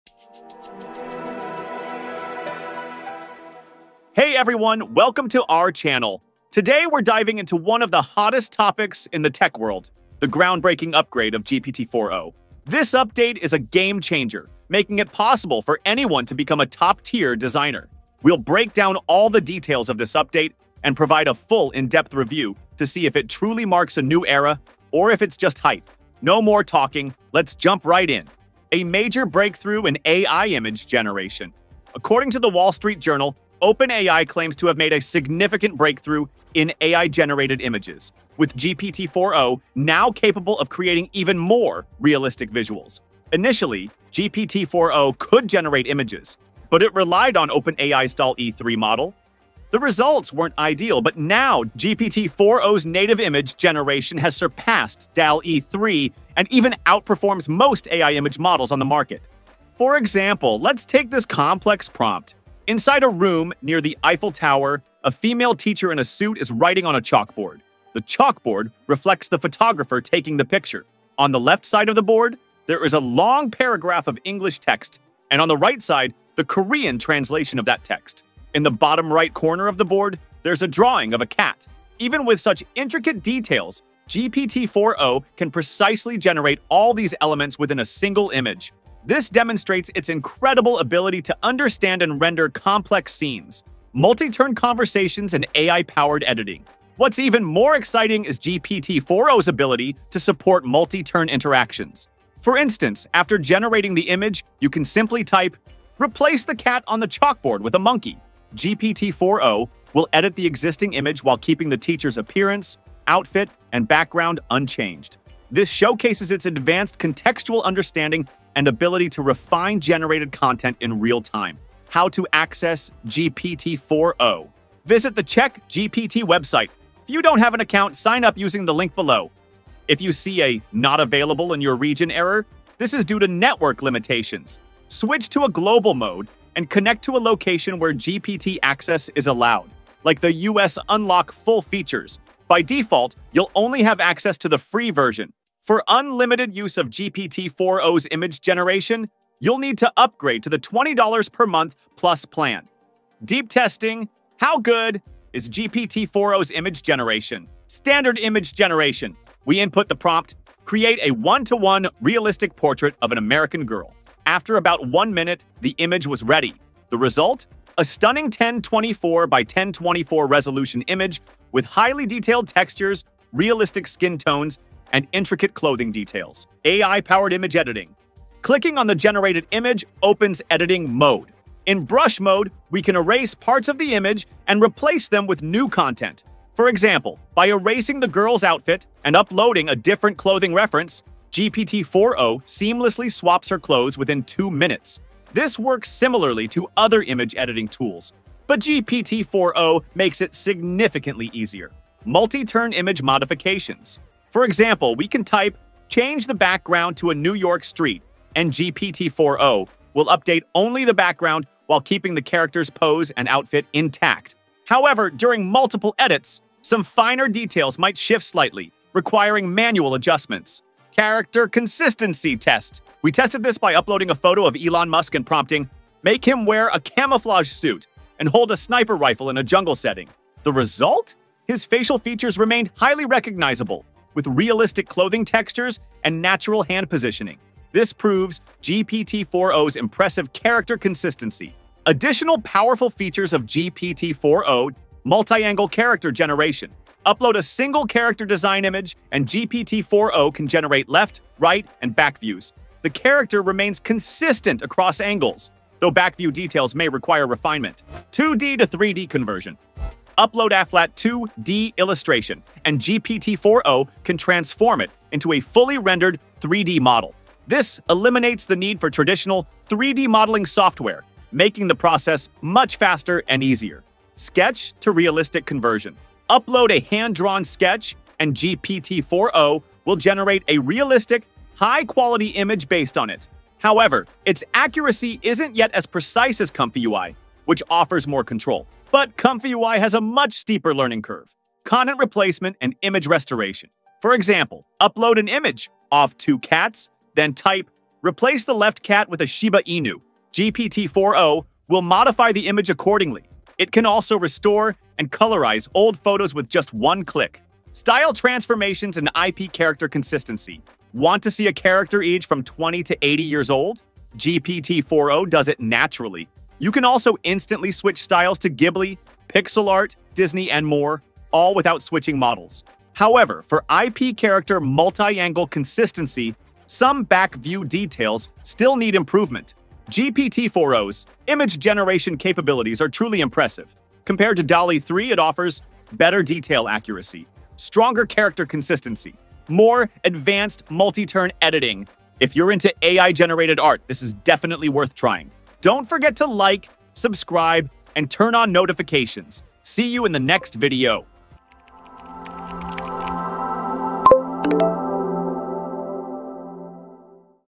GPT-4o Full Review, Everyone Is A Top Designer
Discover how OpenAI’s GPT-4O is revolutionizing AI image generation! 🚀 In this video, we test its groundbreaking features—from creating hyper-detailed scenes (like a Parisian classroom with a cat-drawing chalkboard 🐱) to real-time editing (turning Elon Musk into a jungle sniper!). See how it outperforms DALL-E 3, unlocks 2D-to-3D conversions, and even restores old photos. Is it worth the $20/month upgrade? Watch for our final verdict!

Chapters:
0:00 - Intro: GPT-4O’s Design Revolution
0:29  - Major Breakthrough: Beating DALL-E 3?
1:34 - Multi-Turn Editing: Replace Objects in Seconds!
2:03 - How to Access GPT-4O (Free vs. Plus Plan)
2:34 - Testing Image Quality: Realistic Portraits & Edits
4:02 - Bonus Features: 2D→3D, Aging Characters, Style Swaps
5:08 - Final Verdict: Should YOU Upgrade?